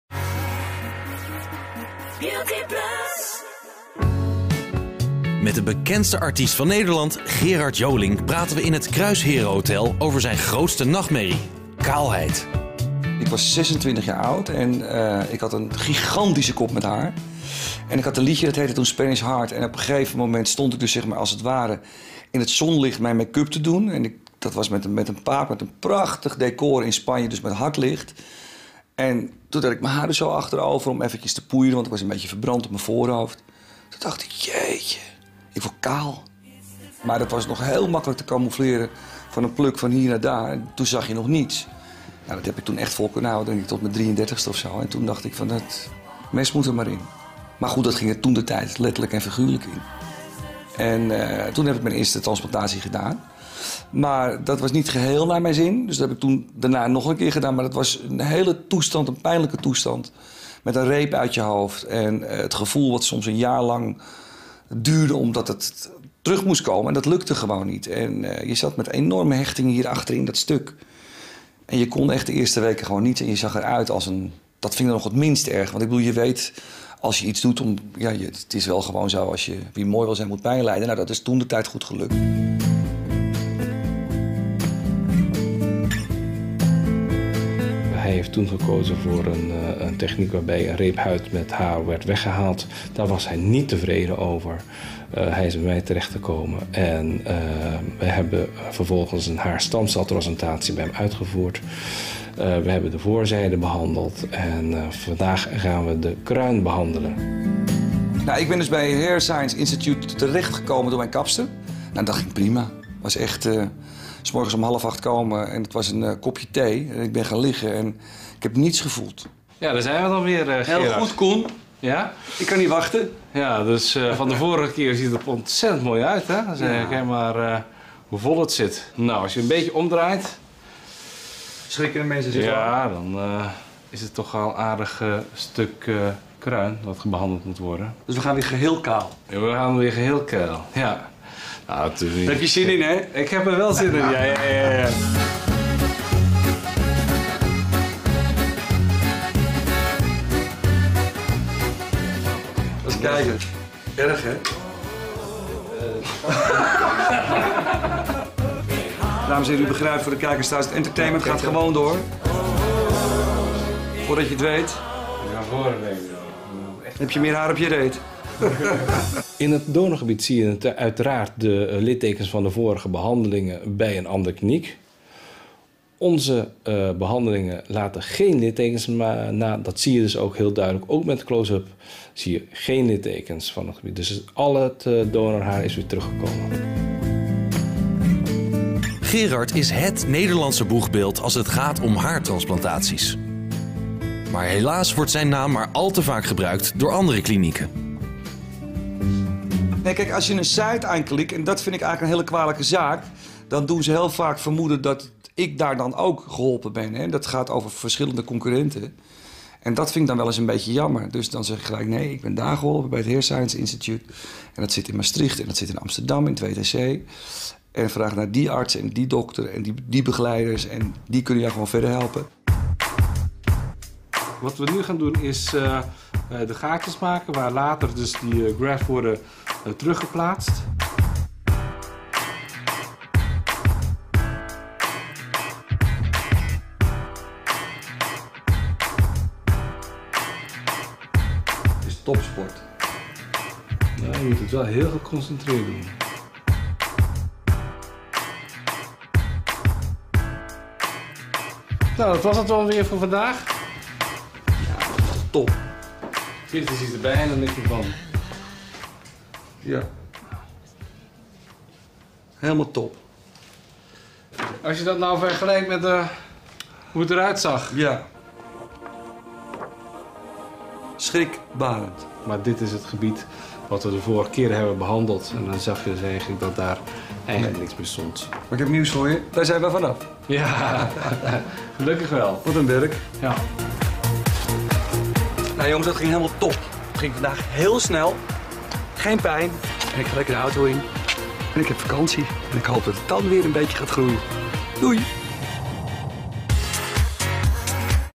Archief: Gerard Joling kiest voor de unieke HaarStamcel Transplantatie methode
HaarStamcel Transplantatie (HST) biedt bij veel vormen van kaalheid een definitieve oplossing. Of het nu gaat om dun haar, wenkbrauwen of (bij mannen) diepe inhammen, een kalende kruin of kale plekken in je baard.

Bijzonder aan deze gepatenteerde haartransplantatie techniek is dat slechts een klein deel van de haarfollikel – en dus niet het hélé haarzakje – wordt geoogst uit het donorgebied. Dat minuscule stukje weefsel bevat voldoende stamcellen om in het ontvangstgebied nieuw haar te doen groeien. Óók in het donorgebied groeit het haar terug.